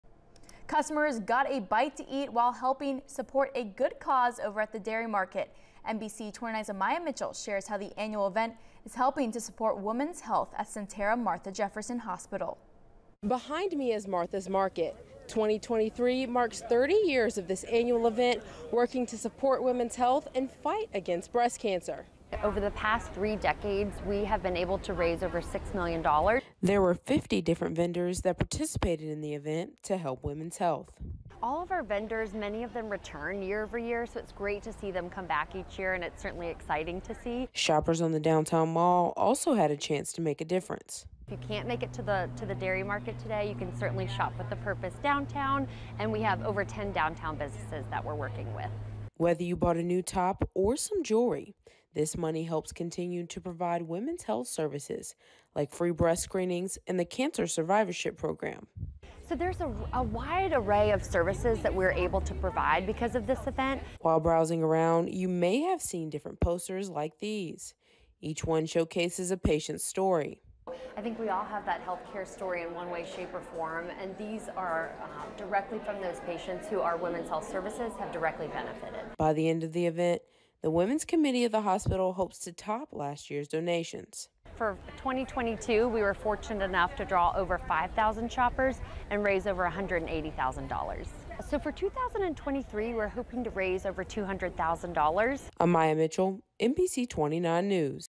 Martha’s Market returns to raise money for women’s health
2023 marks 30 years of Martha’s Market, an annual event that works to support women’s health and fight against breast cancer.